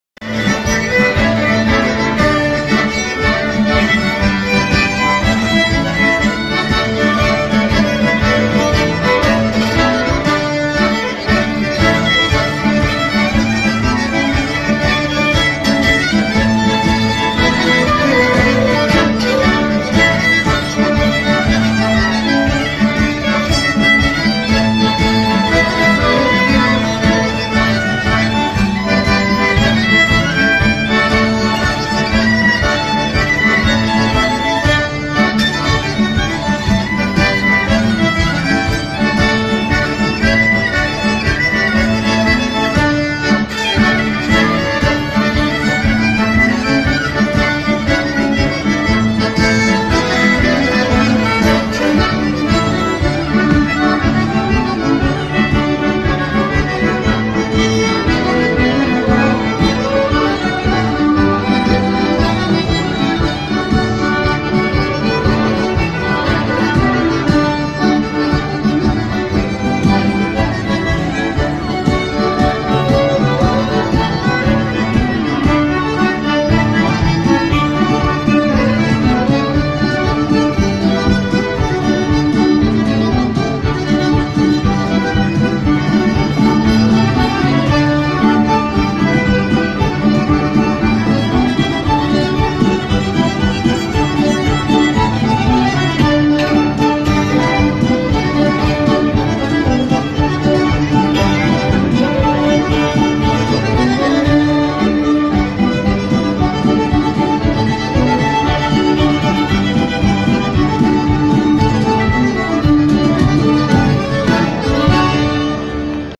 Giga, con Dina Staro e i Suonatori del Savena, special guest Riccardo Tesi
video courtesy by Ruth Widmer-Battilani
notte della tresca,quarta edizione, imola in musica 2017